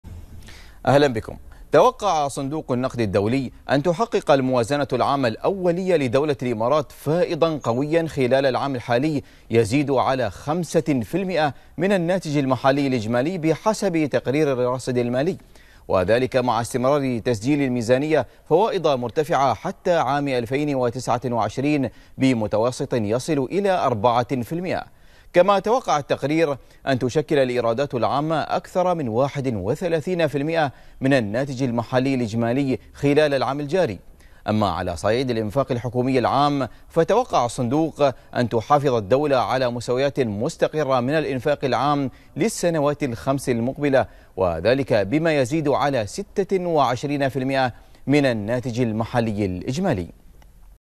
صندوق النقد الدولي يتوقع فوائض قوية في موازنة الإمارات حتى عام 2029
تداول